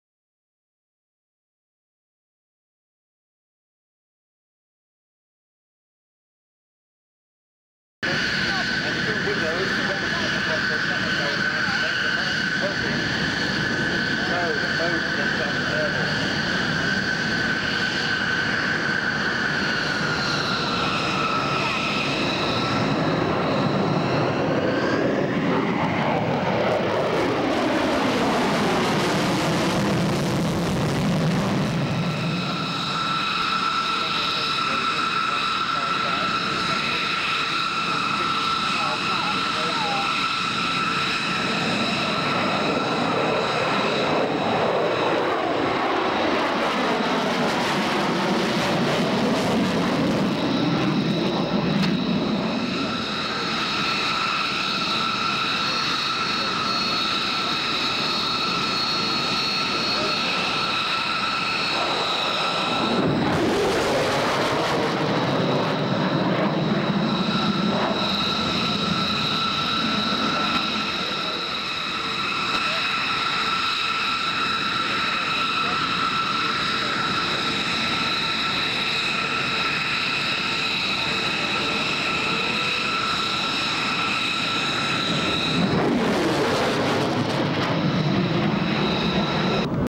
B-1B Lancer High Speed Low Level MilitaryAircraft.de
Super fast Rockwell B-1B Lancer heavy long-range bomber at RAF Mildenhall Air Fete 2000. Very rare airshow footage of the "Bone" performing some high speed passes at more than 900 km/h and very low altitude.